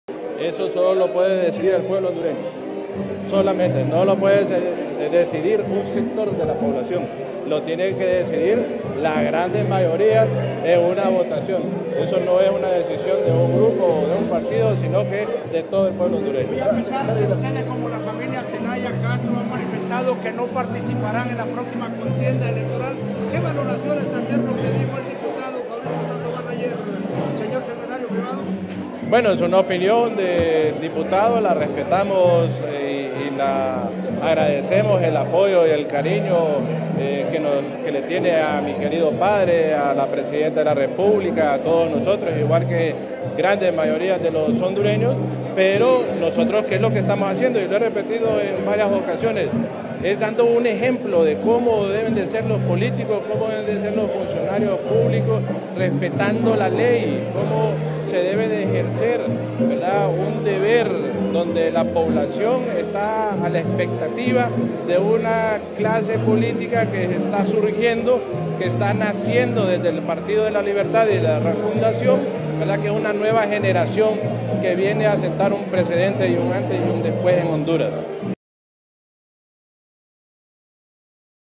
La familia Zelaya no participa en las elecciones, explica Héctor Zelaya
“Es dando un ejemplo de cómo deben de ser los políticos, respetando la ley”, El secretario privado Héctor Zelaya, explicó la razón del porqué la familia Zelaya no participará en las próximas elecciones.